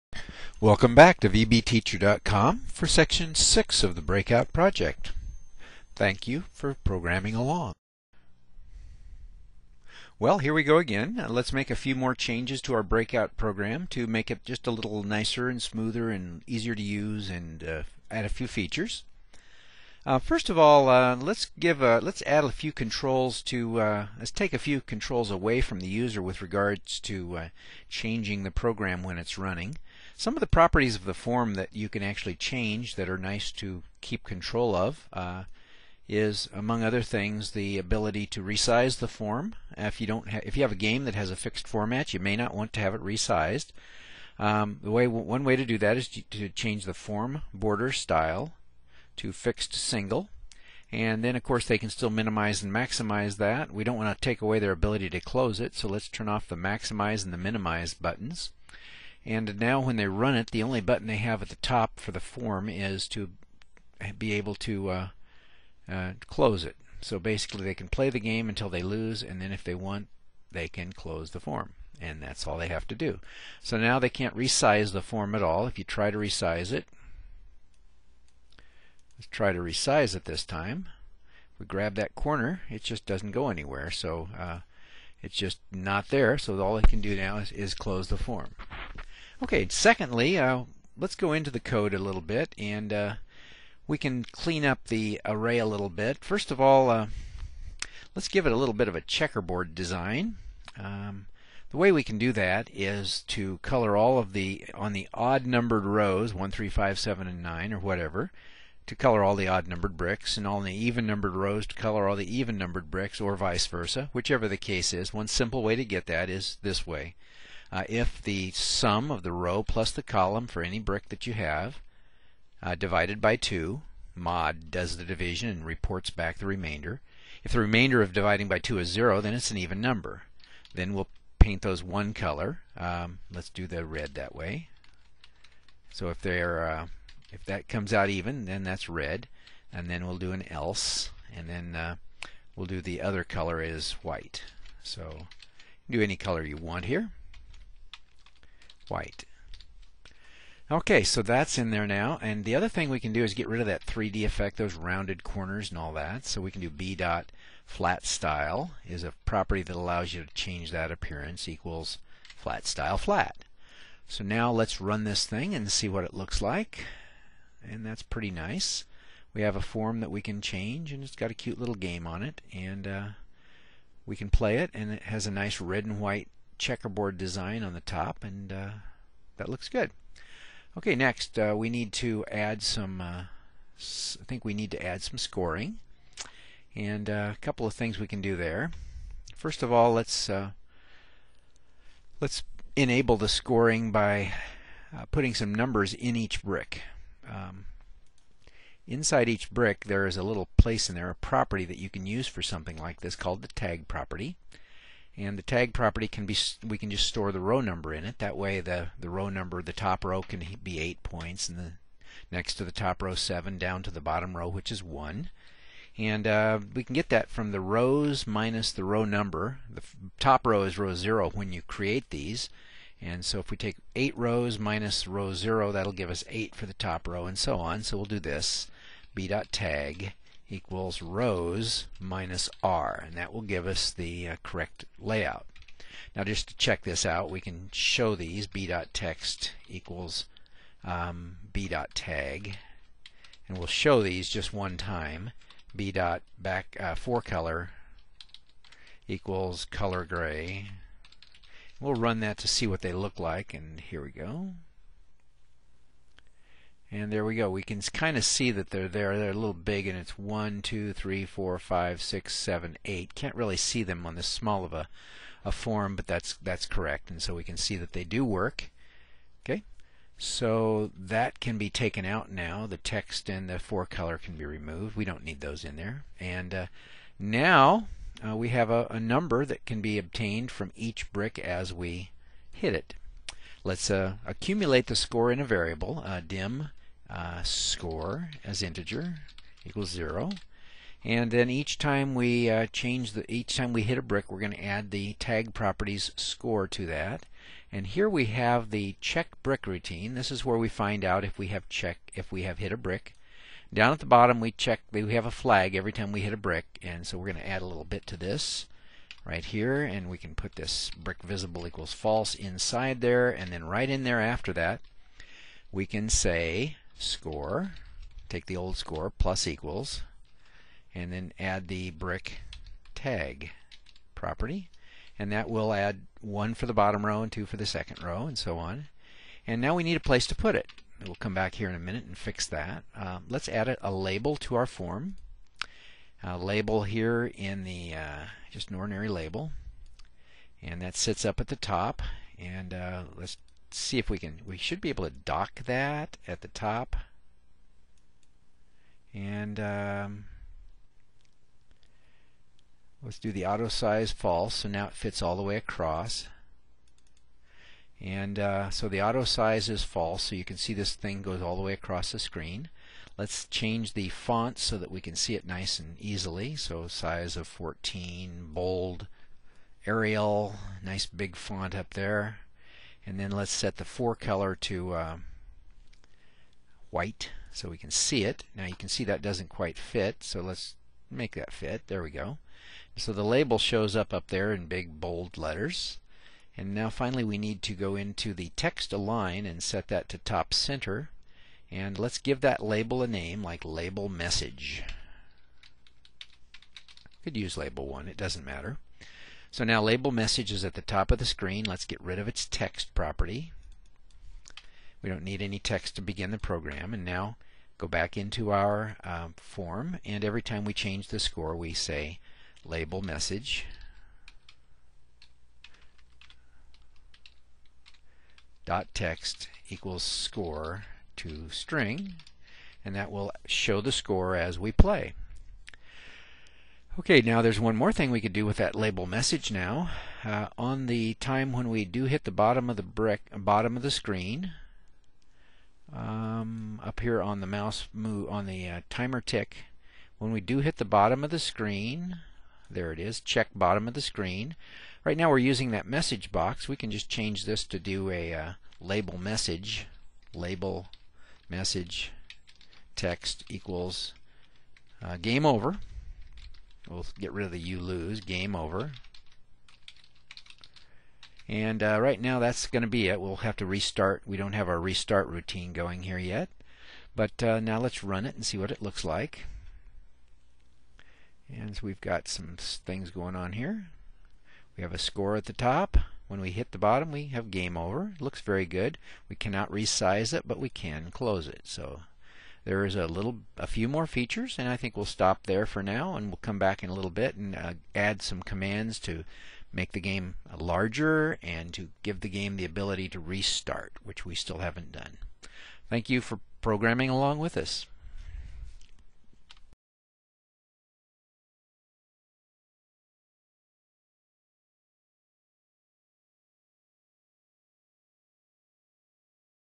Breakout 6 of 7 - Visual Basic Game
Sixth in a series of seven tutorial videos for beginners on learning to program in Visual Basic.  In this tutorial, we begin add some simple scoring options.